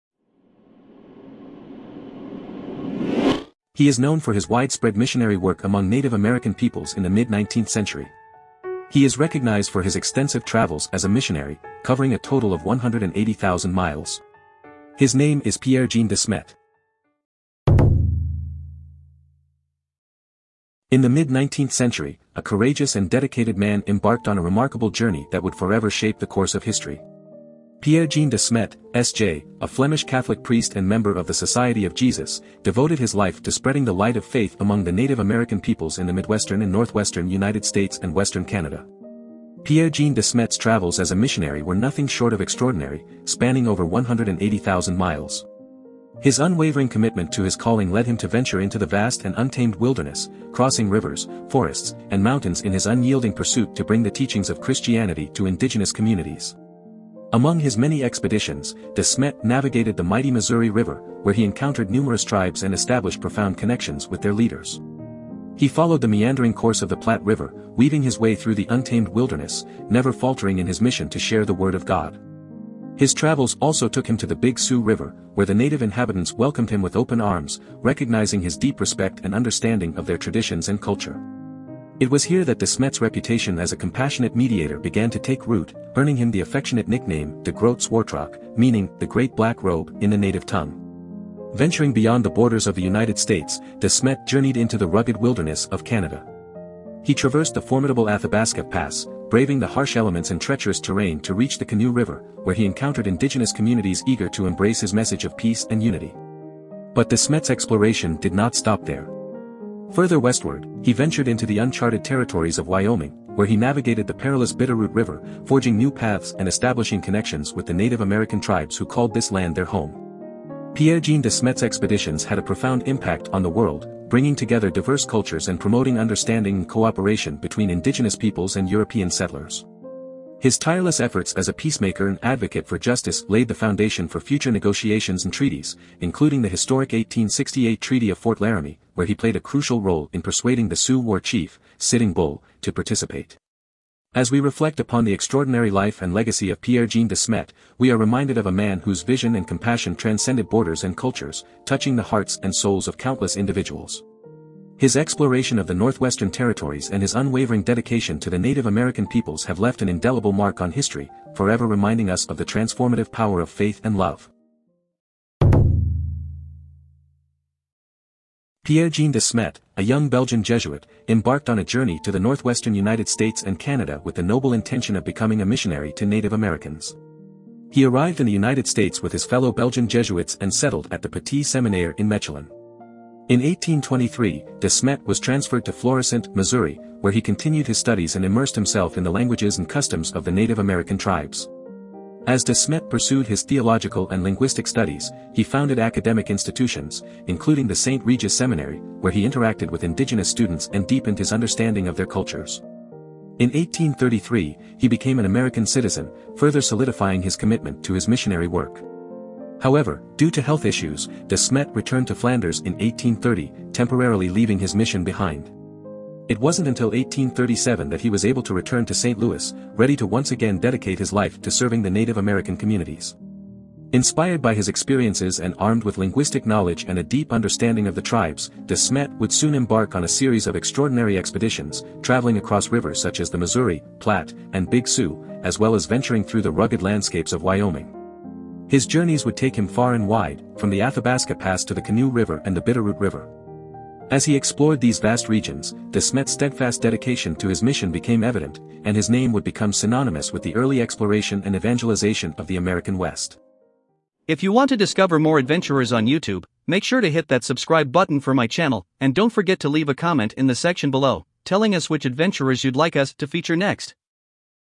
The Revered Missionary: Pierre-Jean De Smet | Explorer Biography
Pierre-Jean De Smet, SJ , also known as Pieter-Jan De Smet, was a Flemish Catholic……